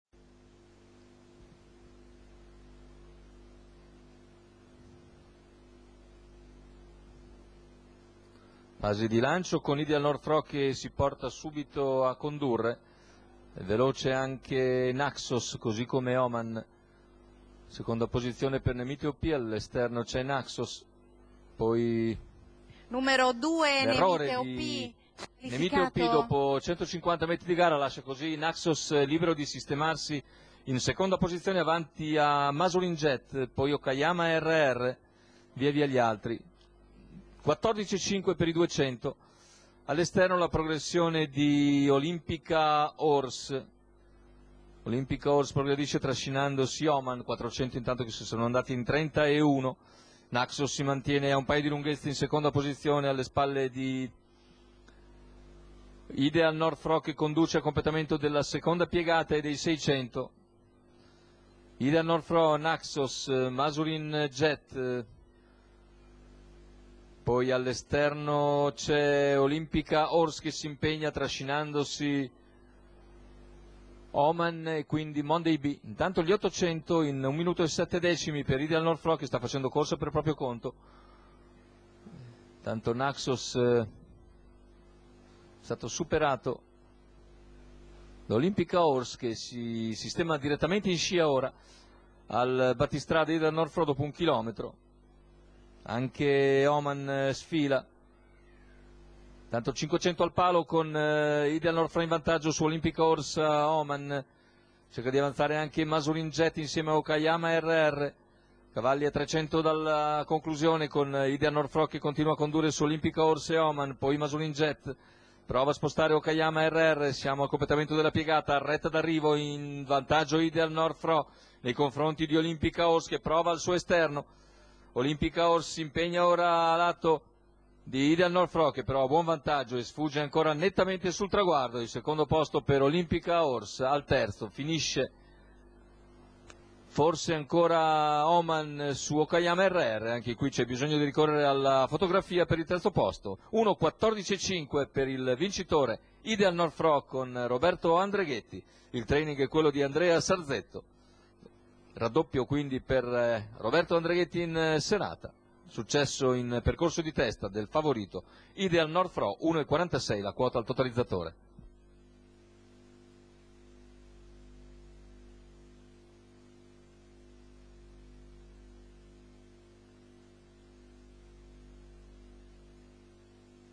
20/07/2013, Cesena , Premio Mps Manif.porc.sar.
1. IDEAL NORD FRO (ANDREGHETTI ROBERTO), 2. OLIMPICA ORS (PORZIO G. JR), 3. OMAN (STEFANI M.), 4. OKAYAMA RR (BUZZITTA A.), 5. MASURIN JET (VITAGLIANO AND.), 6. MONDAY BI (DI NARDO A.), 99. NEMITE OP (LUONGO V.), 99. NAXOS (D' AMBRUOSO GIANVITO)